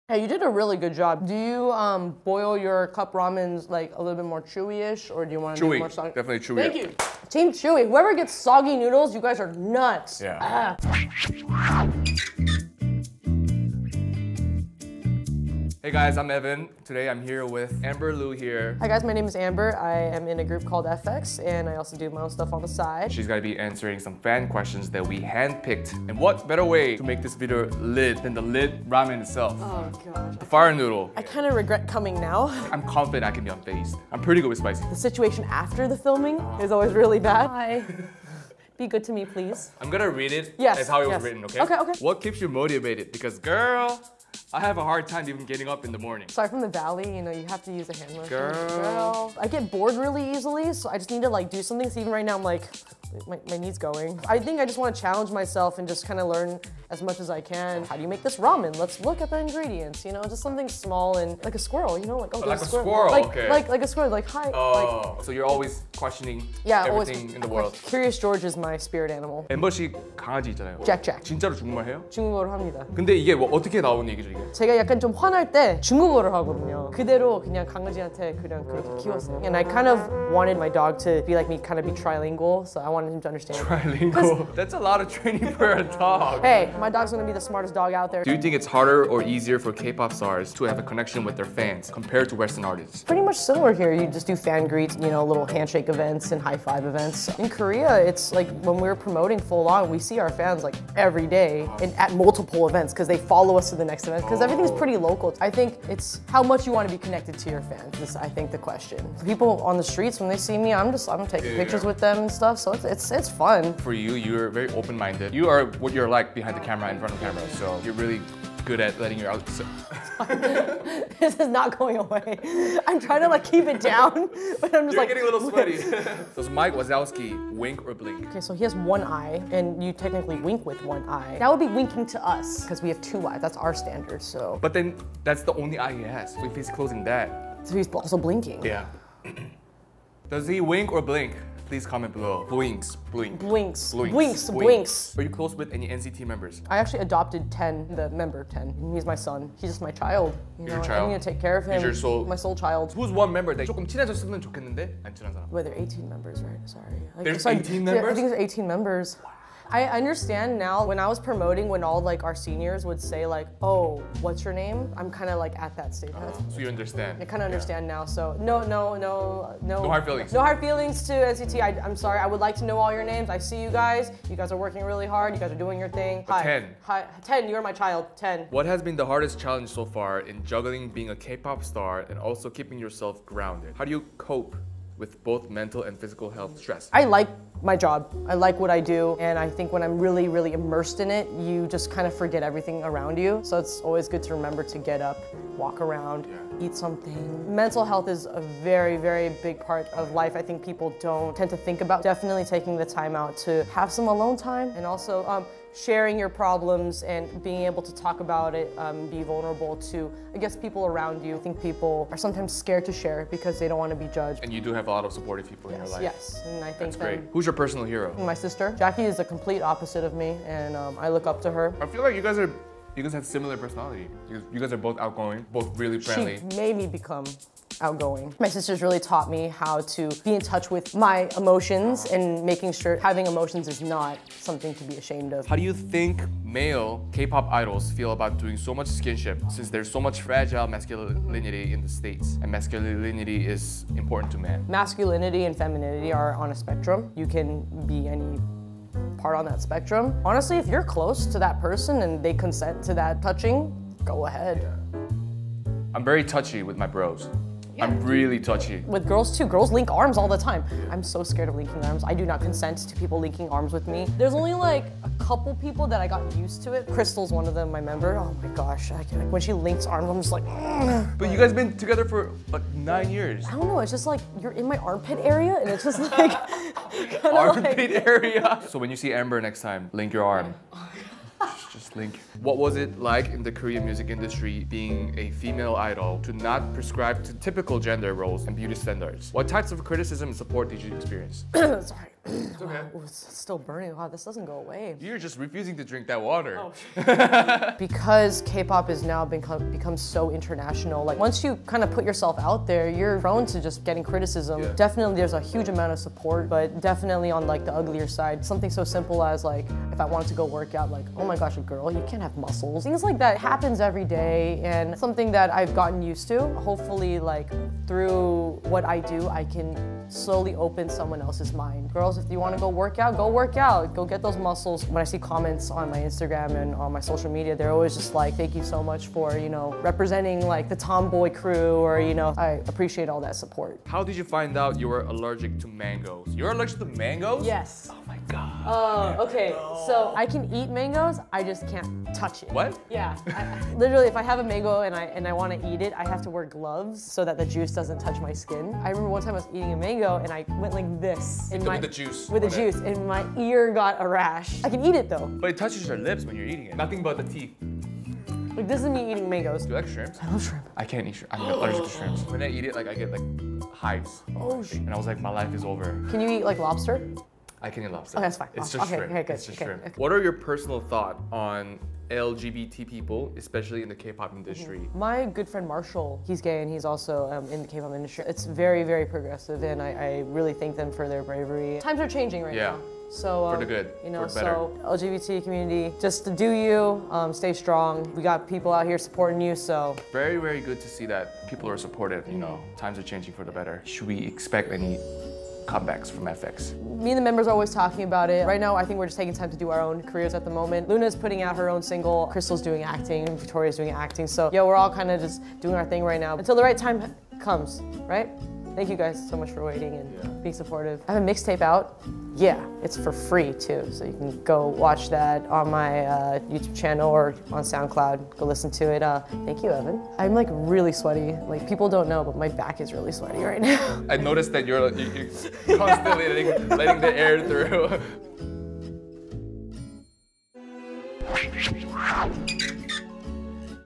Amber Liu Answers Fan Questions While Eating Fire Noodles
Chewy or soggy?

STILLS
The 28th Golden Disk Awards
Choi Soo-Young/Getty Images
Fire Flames
bonezboyz/Getty Images
'Science Behind Pixar' Exhibit
Boston Globe/Getty Images
Blanc&Eclare Launching Event
ilgan Sports/Getty Images

Burning bonfire embers flying up into the night sky
Spencer_Whalen/Getty Images